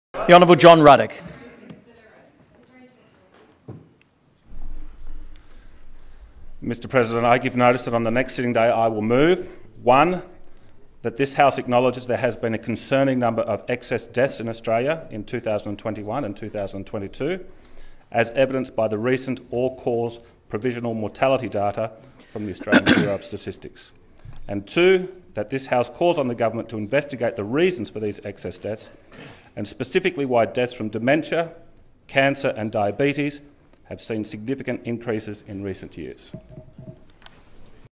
Excess deaths in recent years - John Ruddick MLC calls on the govt to investigate
John Ruddick has given notice that he will put a motion to the chamber calling on the government to acknowledge and investigate the rise of excess deaths in 2021 and 2022.

Liberal Democrats MLC John Ruddick serves in the Legislative Council of the New South Wales Parliament, you can see more from him here: